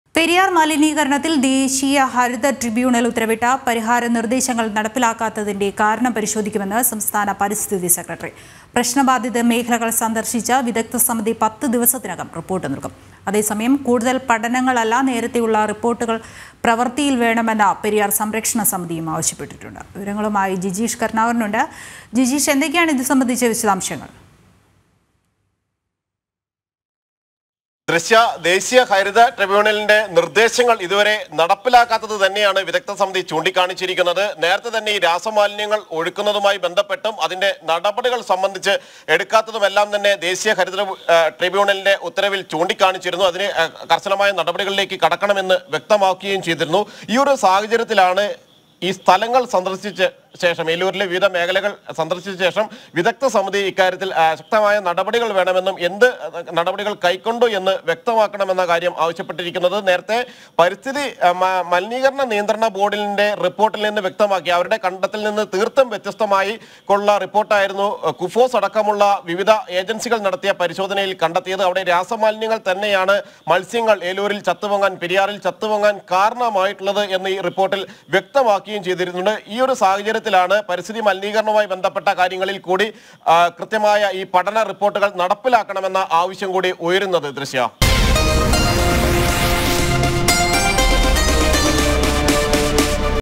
പെരിയാർ മലിനീകരണം, ഹരിത ട്രൈബ്യൂണലിൻ്റെ പരിഹാരങ്ങൾ നടപ്പാക്കാത്തതിന്‍റെ കാരണം പരിശോധിക്കും
പെരിയാർ മലിനീകരണത്തിൽ ദേശീയഹരിത ട്രൈബ്യൂണൽ ഉത്തരവിട്ട പരിഹാര നിർദ്ദേശങ്ങൾ നടപ്പിലാക്കാത്തതിന്‍റെ  കാരണം പരിശോധിക്കുമെന്ന് സംസ്ഥാന പരിസ്ഥിതി സെക്രട്ടറി. പ്രശ്നബാധിത മേഖലകൾ സന്ദർശിച്ച് വിദഗ്ധ സമിതി പത്ത് ദിവസത്തിനകം റിപ്പോർട്ട് നൽകും. അതേസമയം കൂടുതൽ പഠനങ്ങൾ അല്ല നേരത്തെ ഉള്ള റിപ്പോർട്ടുകൾ പ്രവർത്തിയിൽ വേണമെന്ന് പെരിയാർ സംരക്ഷണ സമിതി ആവശ്യപ്പെട്ടു.